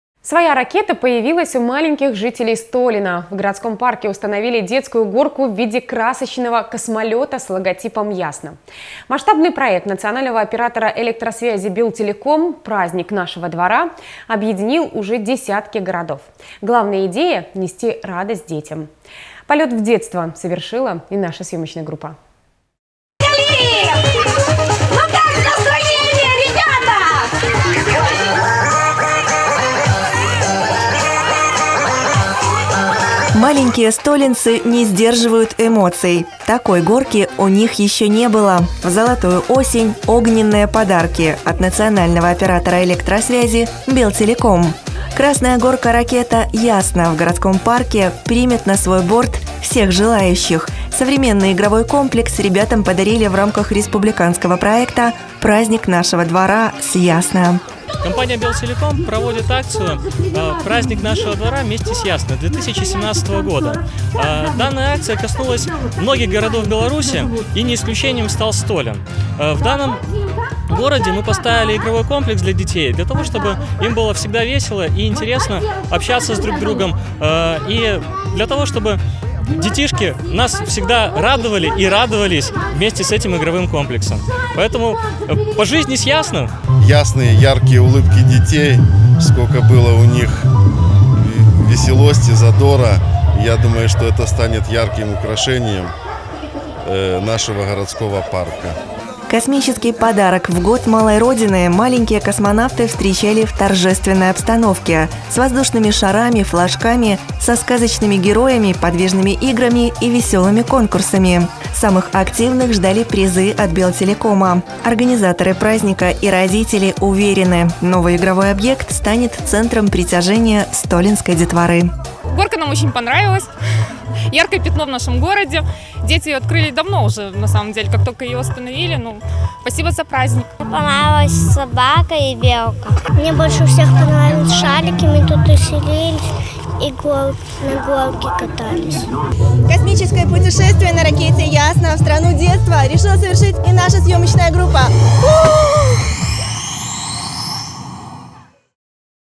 В космос с «Белтелекомом»: в столинском парке установили детскую горку-ракету «ЯСНА»
Своя ракета появилась у маленьких жителей Столина. В городском парке установили детскую горку в виде красочного космолёта с логотипом "ЯСНА". Масштабный проект национального оператора электросвязи "Белтелеком" – "Праздник нашего двора" – объединил уже десятки городов. Главная идея – нести радость детям. Полёт в детство совершила и наша съёмочная группа.